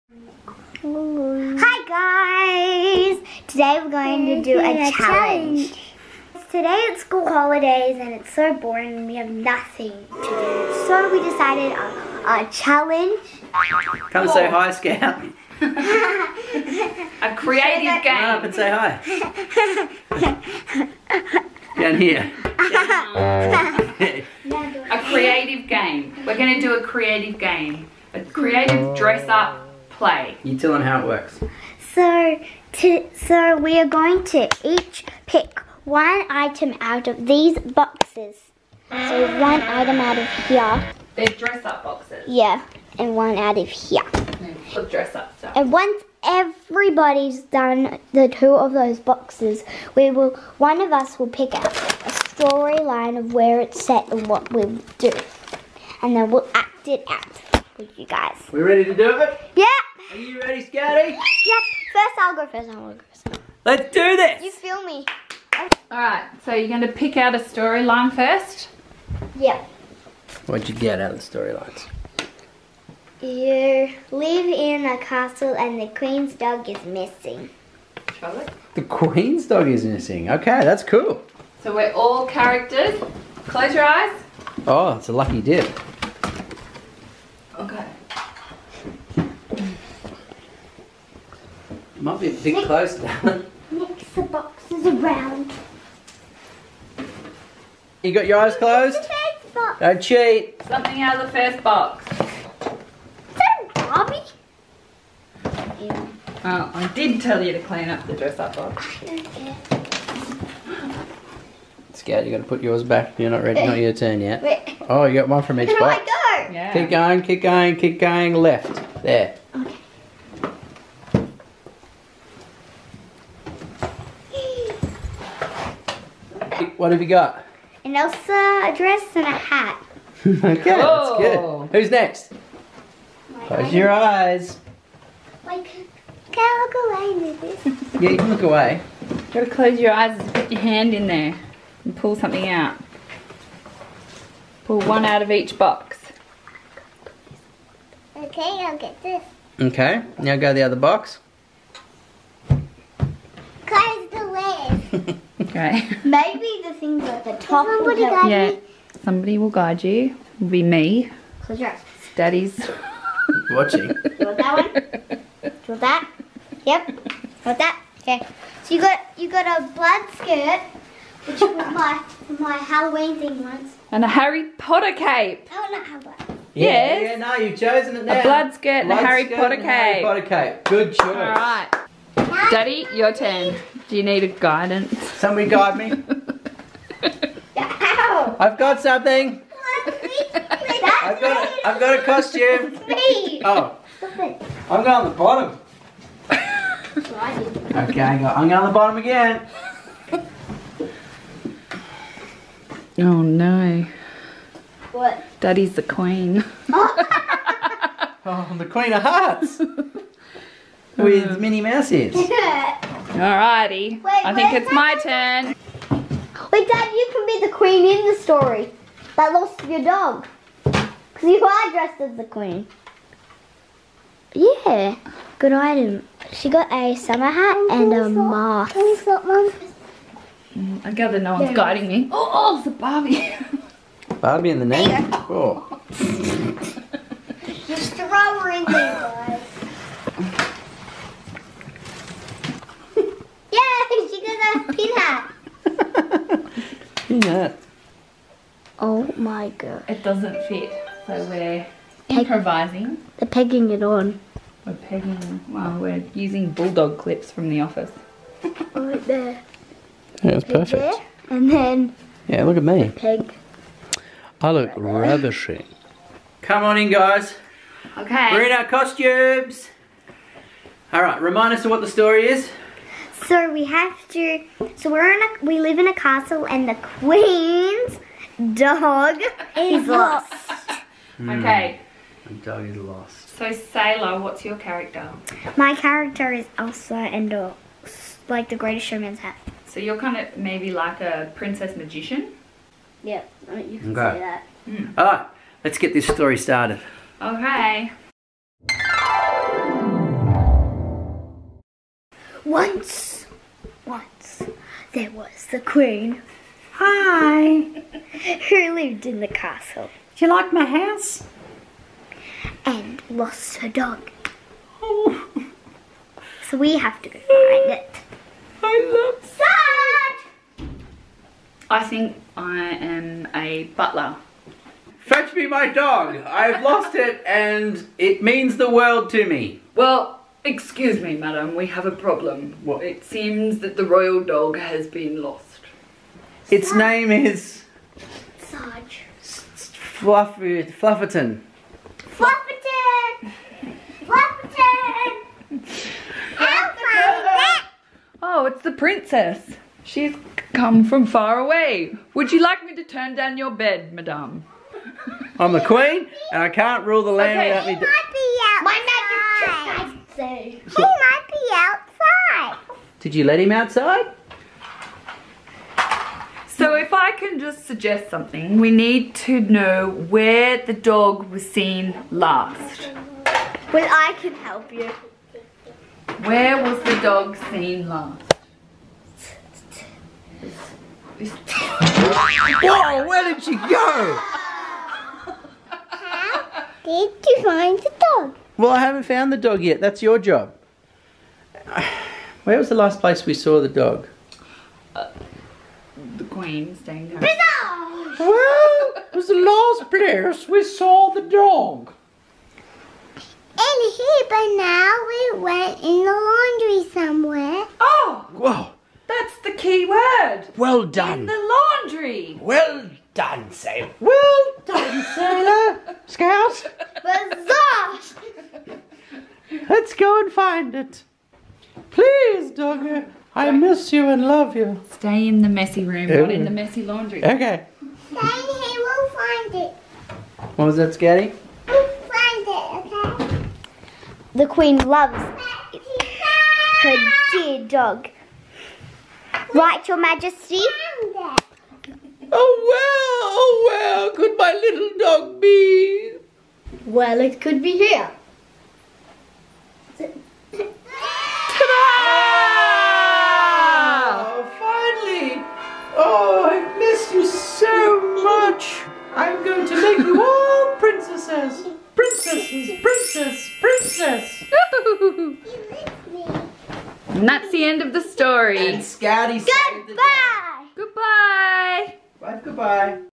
Denyer Holiday Challenge
School holidays can be SOOOOO boring.. So we decided to do a family creative challenge. It's pretty funny. I hope we will do some more of these.
Sailor x